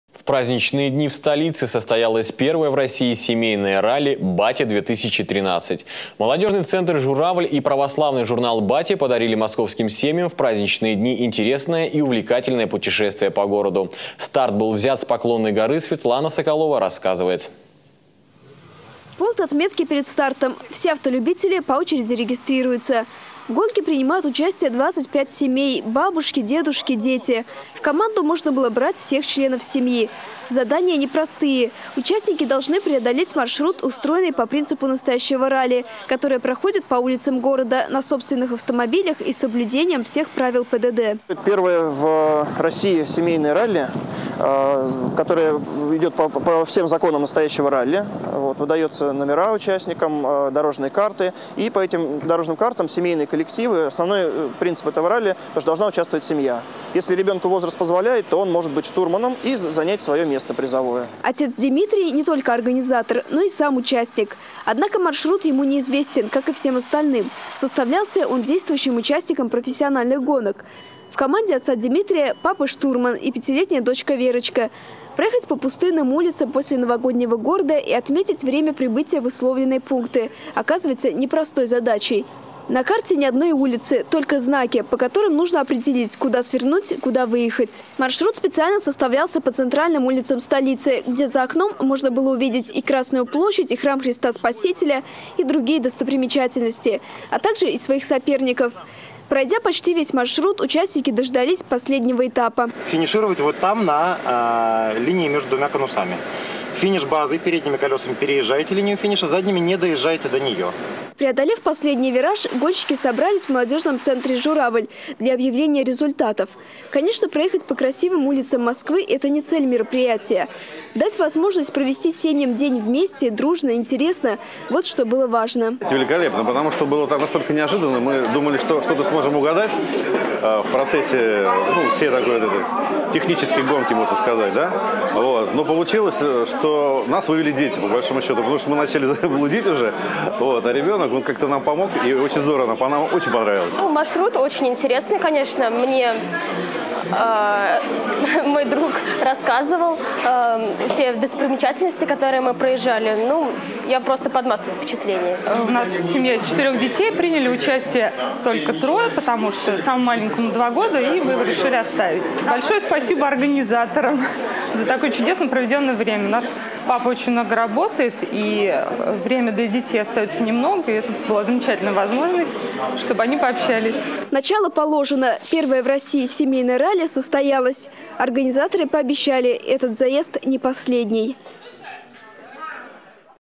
1-е в России семейное ралли «Батя - 2013» - в Москве
В праздничные дни в Столице, состоялось первое в России семейное ралли «Батя - 2013». Молодежный центр «Журавль» и православный журнал «Батя» подарили московским семьям в праздничные дни интересное и увлекательное путешествие по городу. Старт был взят с Поклонной горы.